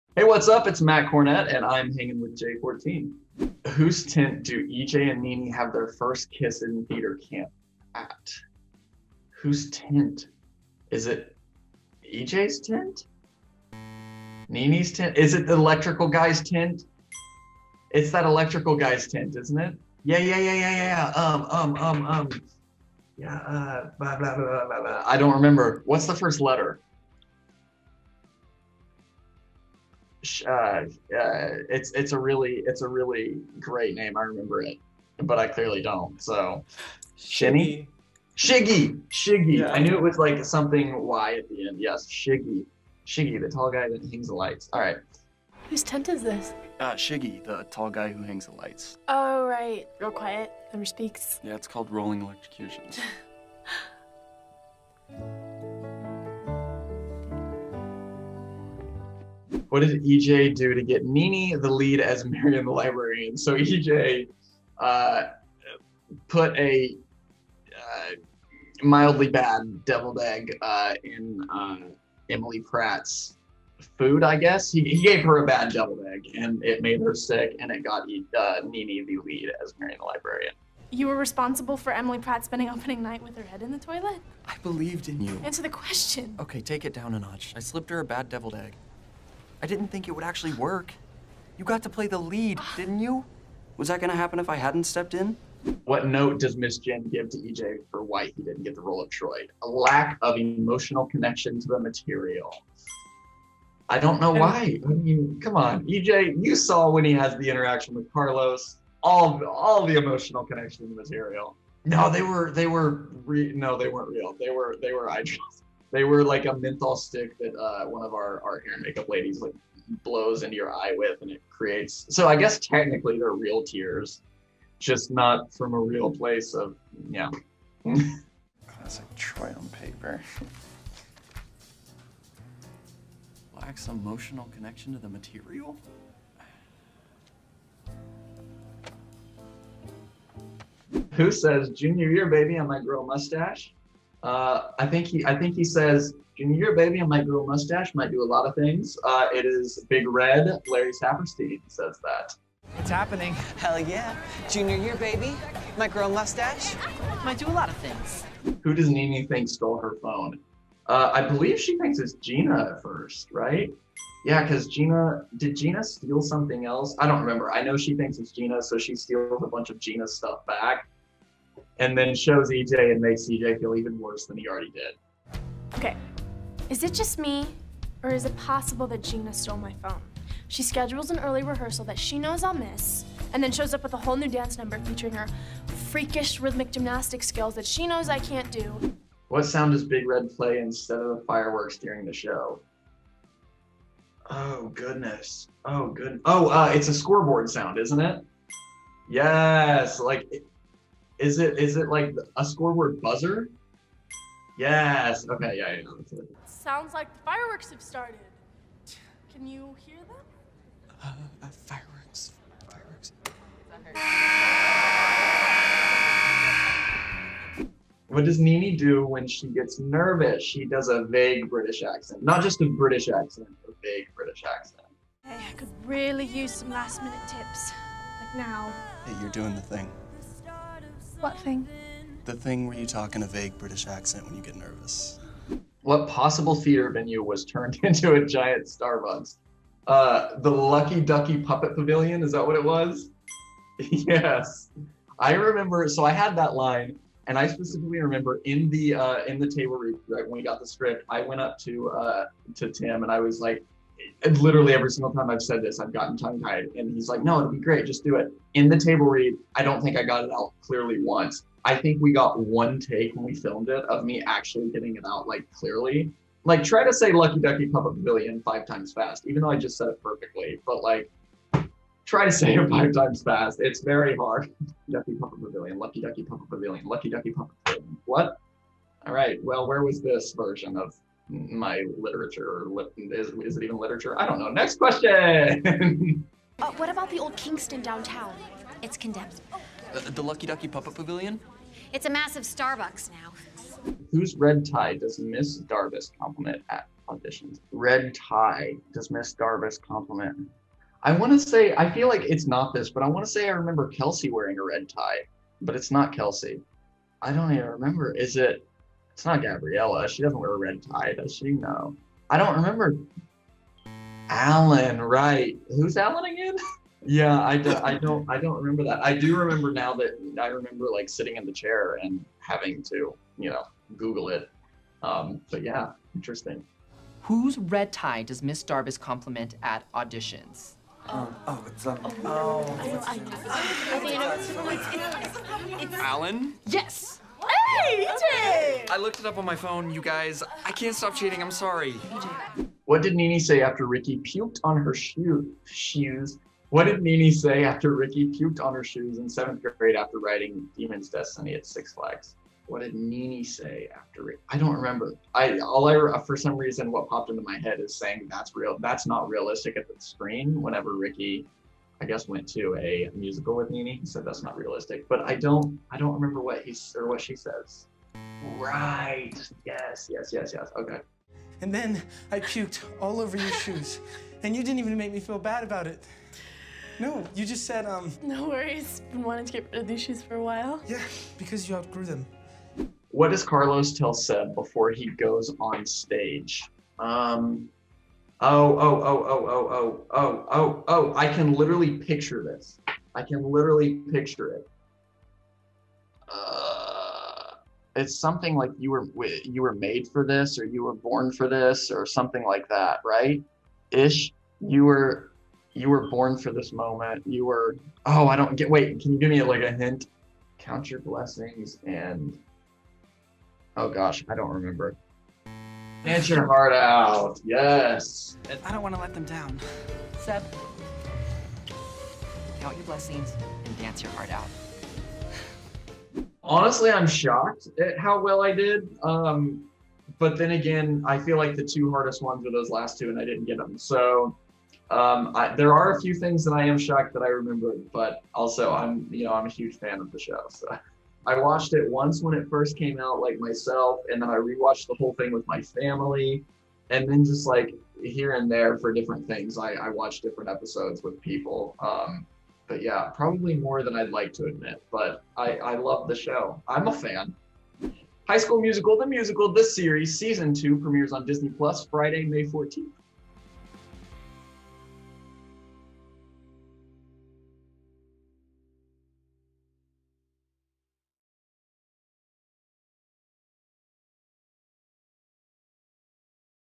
HSMTMTS Trivia With Matt Cornett
How well does Matt Cornett (EJ) know his Disney Plus show High School Musical The Musical The Series? We put him to the ultimate test with super hard trivia questions! See how many he gets right in the video above and let us know how many YOU got right in the comments!

HSMTMTS season 2 premieres Friday, May 14 on Disney+